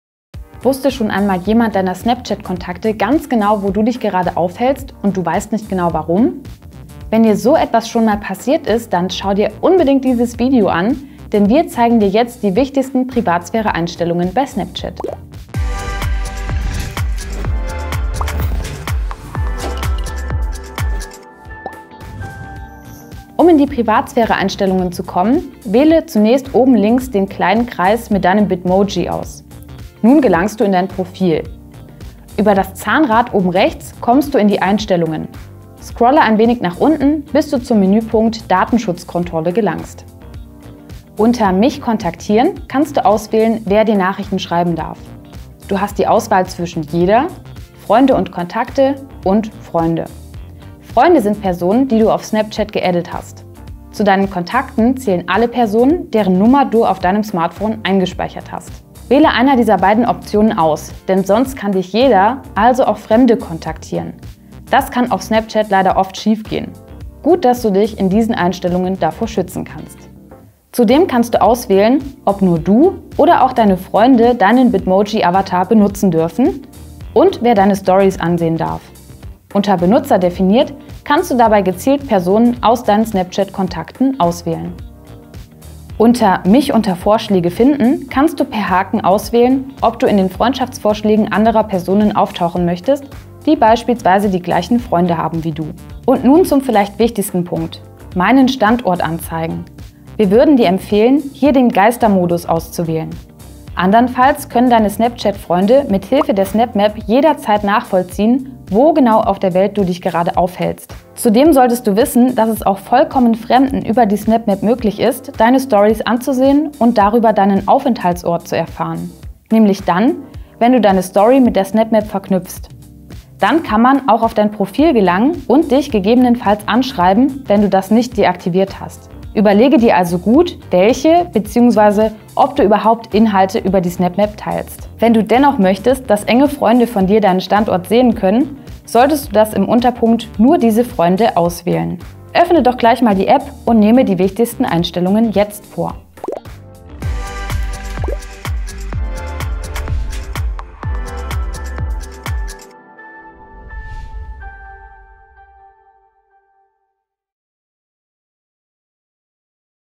Privatsphäre-Einstellungen bei Snapchat: 2023
In diesem Video zeigen wir dir, wie du deine Privatsphäre bei Snapchat schützen kannst!

00:00 Einleitung
00:22 So kommst du in die Einstellungen
00:40 Wer darf dir schreiben?
01:10 Wer darf deinen Avatar benutzen und deine Stories sehen?
01:25 Willst du in Freundschaftsvorschlägen anderer auftauchen?
01:35 Achtung Datenschutzlücke: Bitte Geistmodus aktivieren!

Öffne doch direkt mal deinen Snapchat Account und nehme die wichtigsten Einstellungen vor! :)